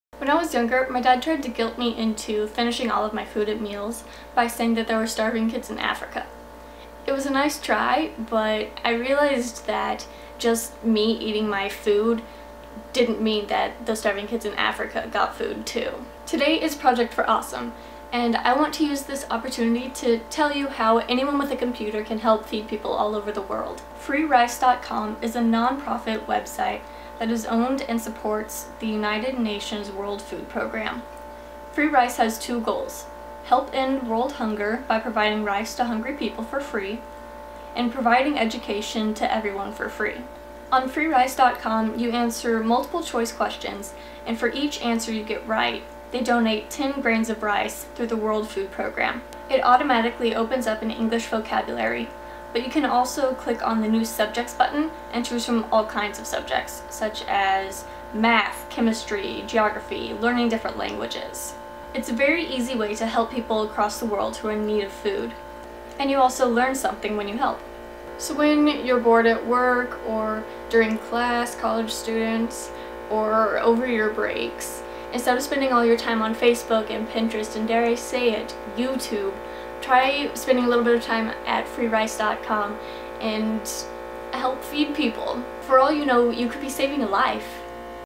P4A 2012 - FreeRice.com
This year for The Project for Awesome I am talking about Freerice.com Check it out. Comment spam a lot so the VlogBrothers will pay a penny for each comment.